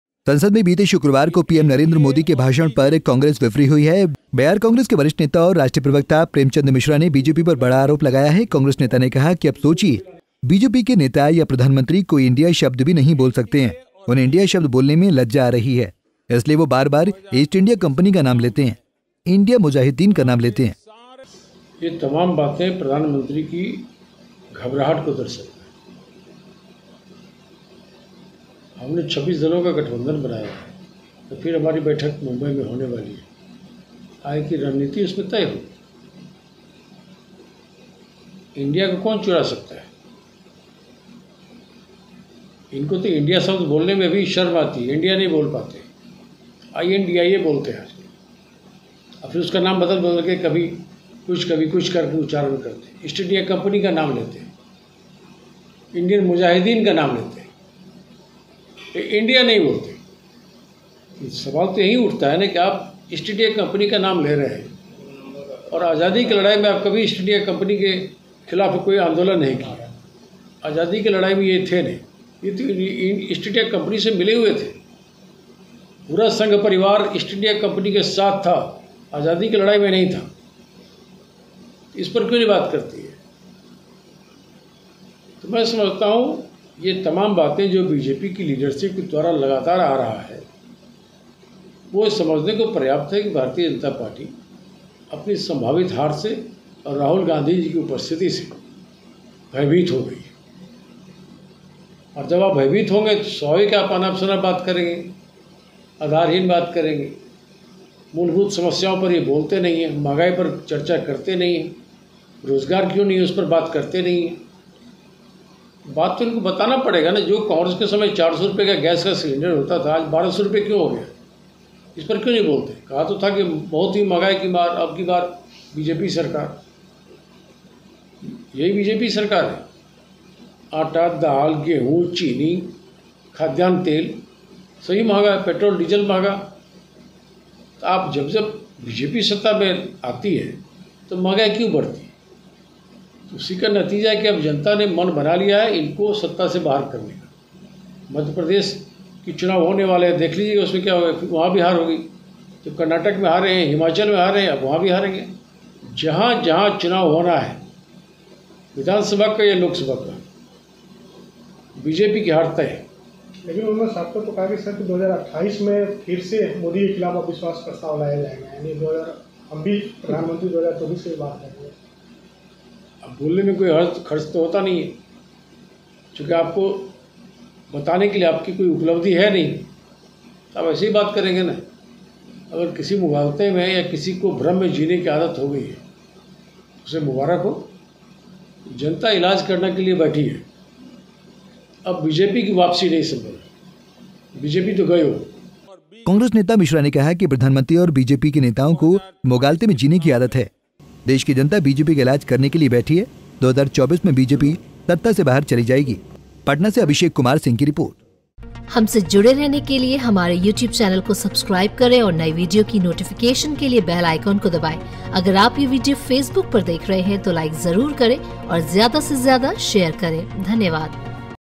Congress ने PM Modi पर लगाया बड़ा आरोप, कहा- इंडिया शब्द बोलने में आती है शर्म
संसद में बीते शुक्रवार को पीएम नरेंद्र मोदी(PM Modi ) के भाषण पर कांग्रेस(Congress) बिफरी हुई है.. बिहार कांग्रेस के वरिष्ठ नेता और राष्ट्रीय प्रवक्ता प्रेमचंद मिश्रा ने बीजेपी बड़ा आरोप लगाया है…